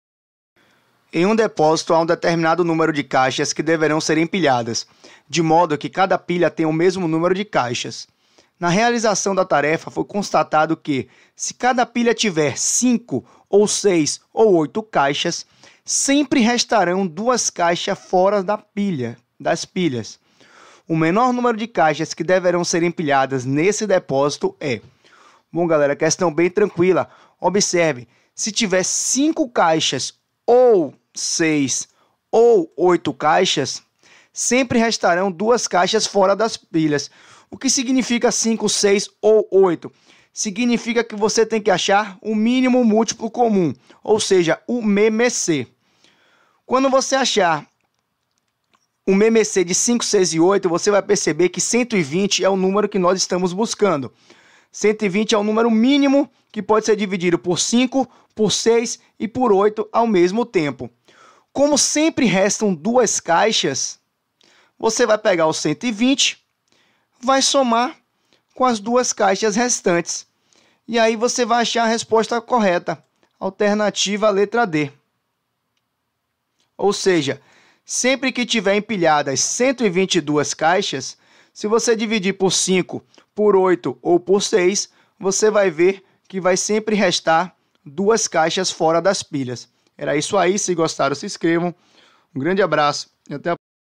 Concurso Vunesp Q.21 - PM SP 2018
Em um depósito há um determinado número de caixas que deverão ser empilhadas, de modo que cada pilha tenha o mesmo número de caixas. Na realização da tarefa foi constatado que, se cada pilha tiver 5 caixas, ou 6 caixas ou 8 caixas, sempre restarão 2 caixas fora das pilhas. O menor número de caixas que deverão ser empilhadas nesse depósito é
(A) 124.
(B) 126.
(D) 122.
(E) 118.